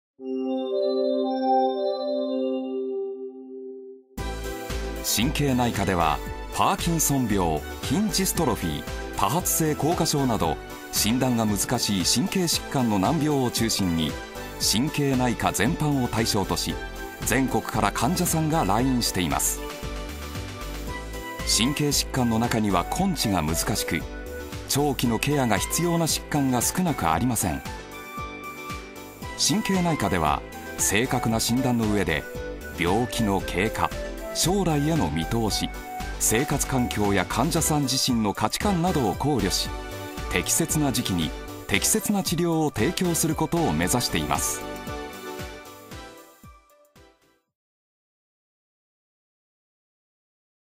NCNP_病院_神経内科
国立精神・神経医療研究センター（略称：NCNP）のYoutube公式チャンネル「NCNPチャンネル」より、NCNP病院の紹介動画―神経内科―です。